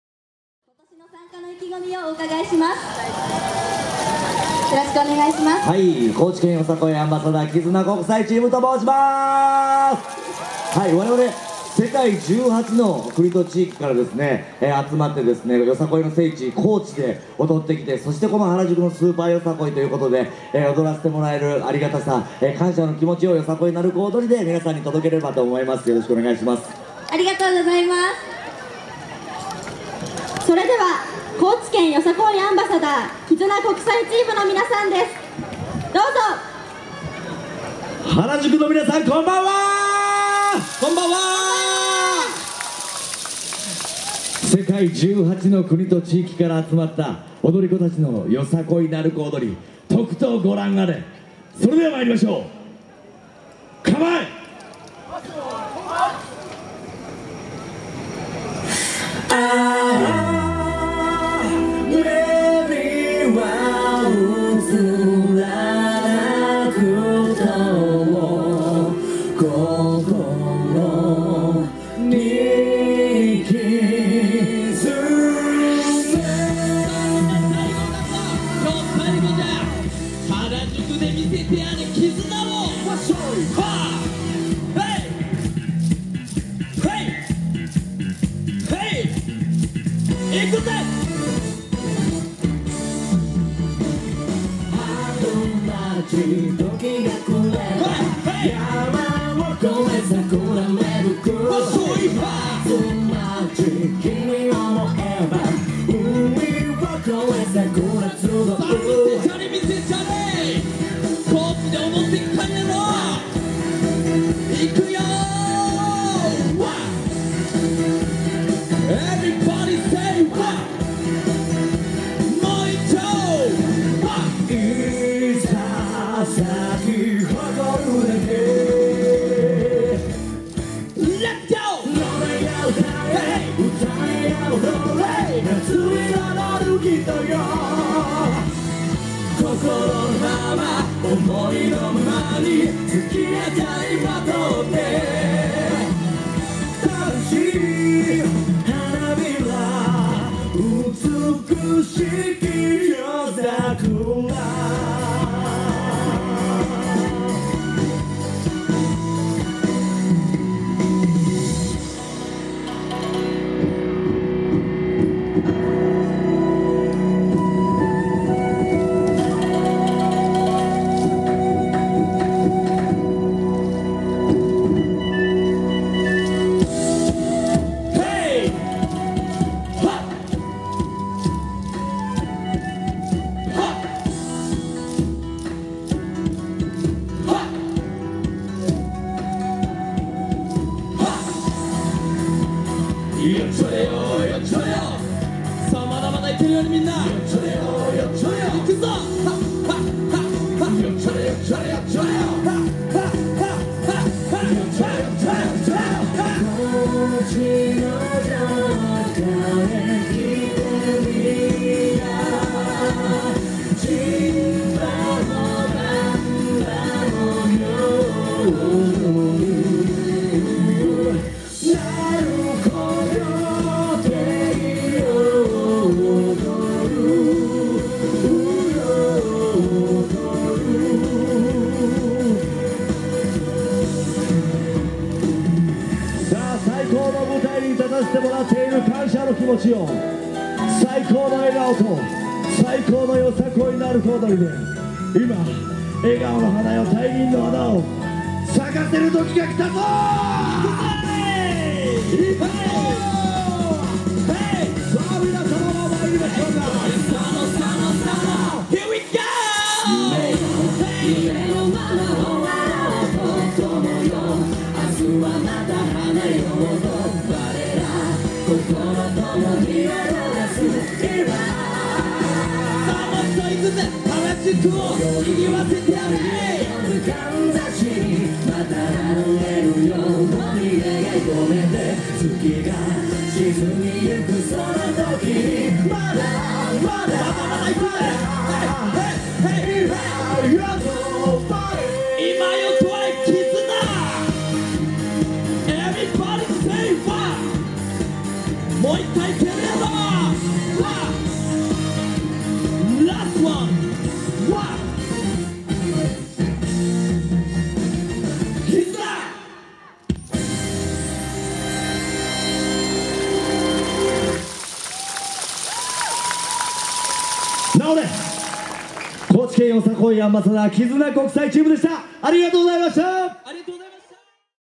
高知県よさこいアンバサダー絆国際チーム 原宿表参道元氣祭スーパーよさこい2019 原宿口ステージ8月24日 / Kizuna International Team, Aug 24th 2019
原宿表参道元氣祭スーパーよさこい2019
高知県よさこいアンバサダー絆国際チーム
原宿口ステージ8月24日
Kochi Yosakoi Ambassador KIZUNA International Team
Harajuku Omotesando Super Yosakoi 2019
Harajuku stage on Aug 24th, 2019